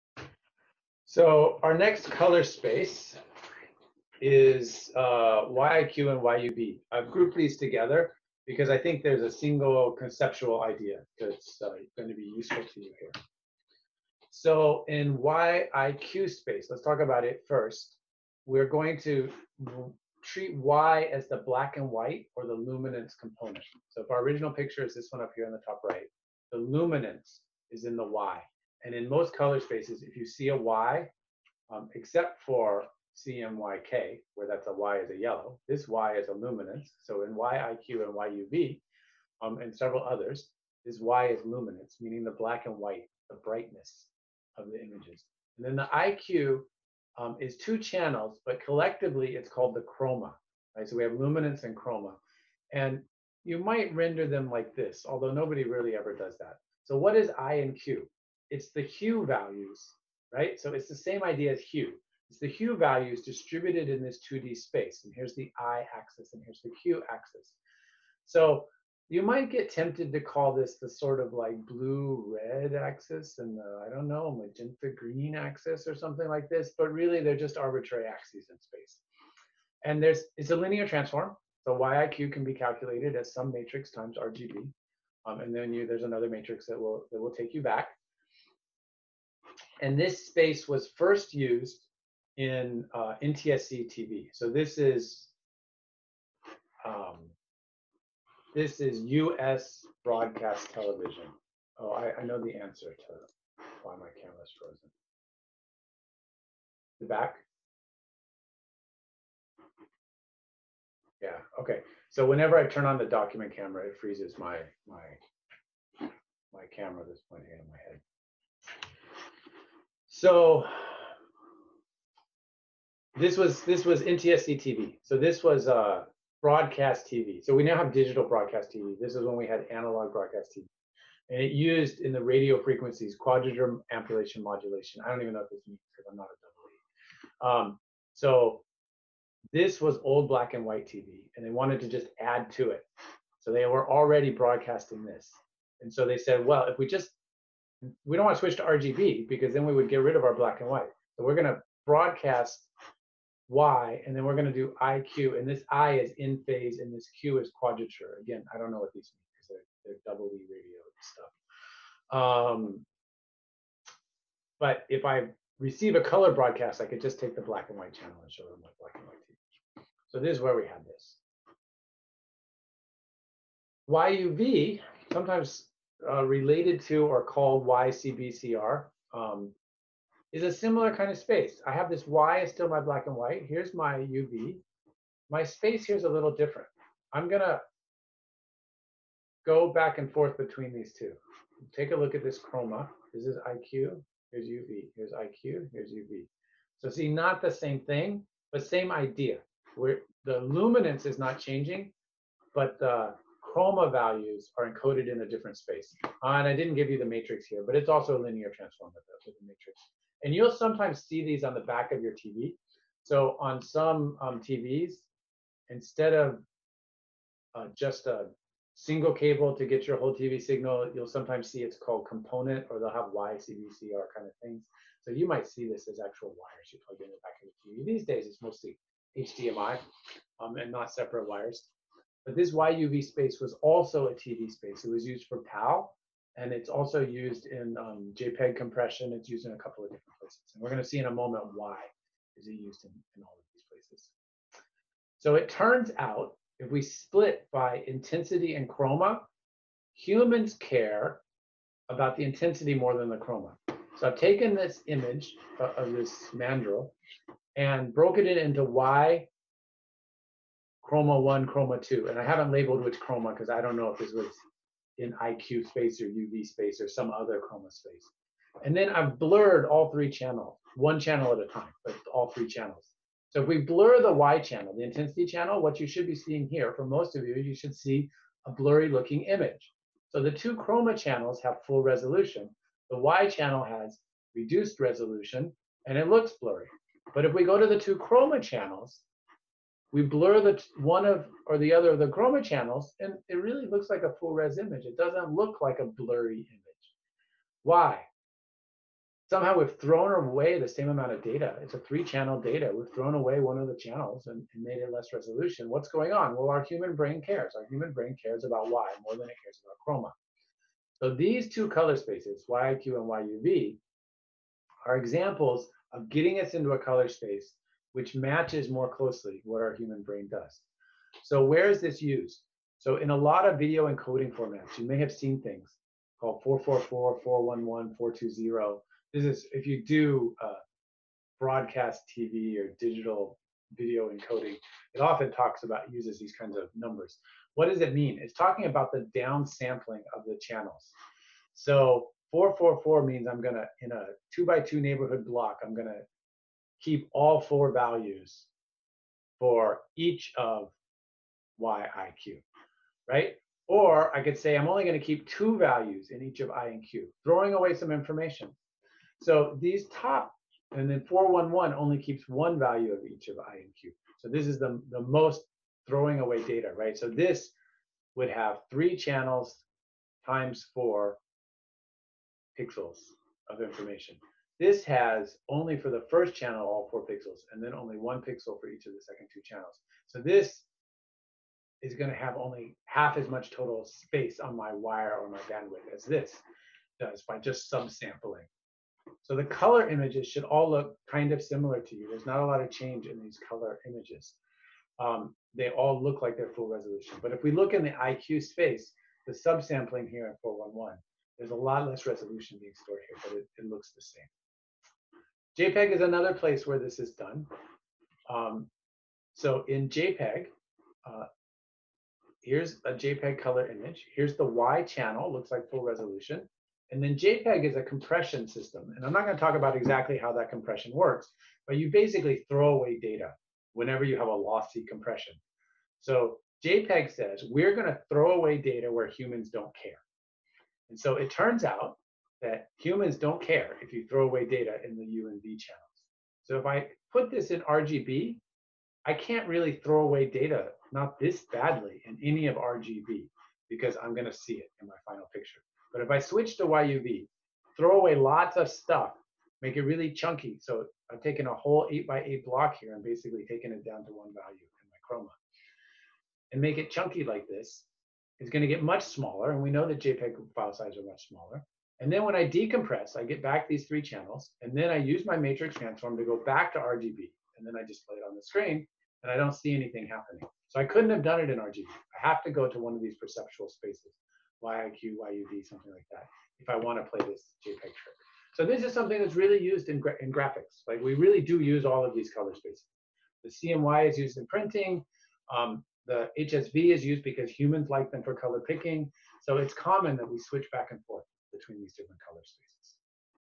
YIQ YUV Color Space
Lesson on YIQ and YUV Color Space taken from the Color lecture conducted  on 4/21/20.